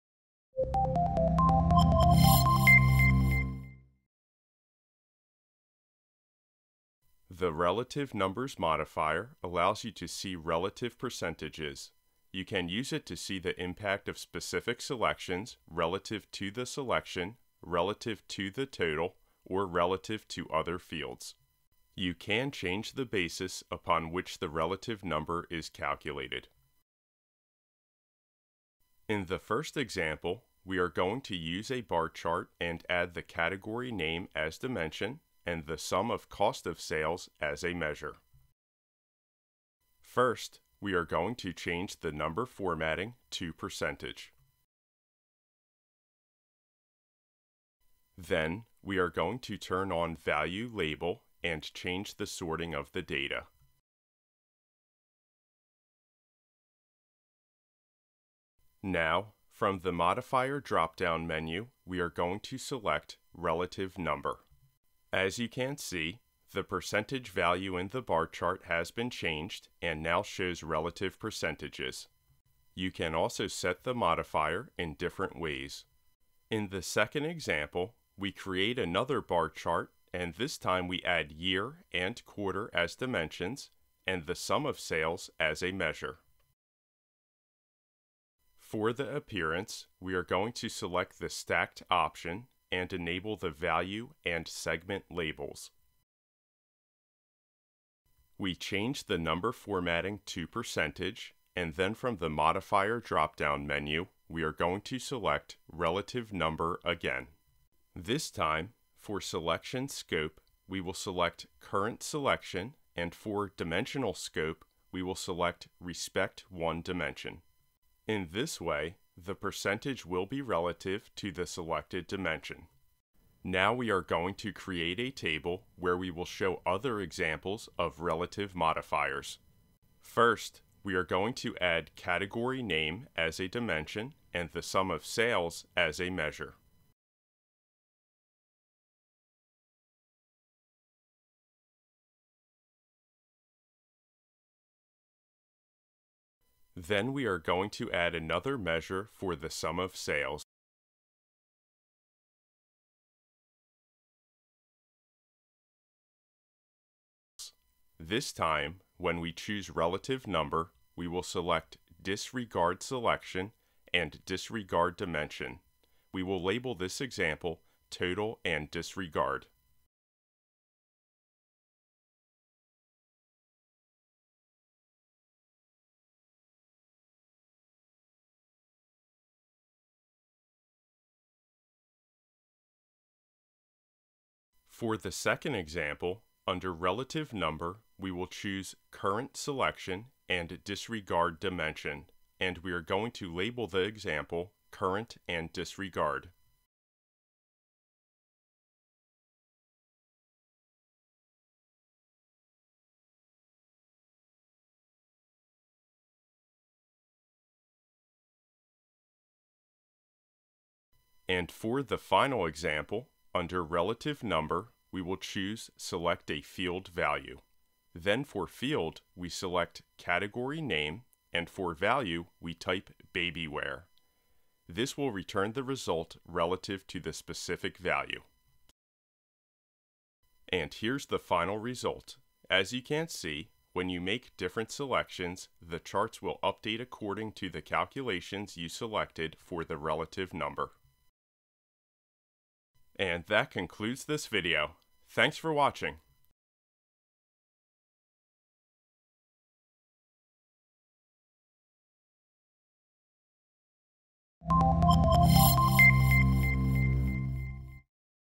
Using relative numbers - Qlik Sense
This videos shows you how to work with the relative numbers. The relative numbers modifier allows you to see relative percentages. You can use it to see the impact of specific selections, relative to the selection, relative to the total, or relative to other fields. You can change the basis upon which the relative number is calculated.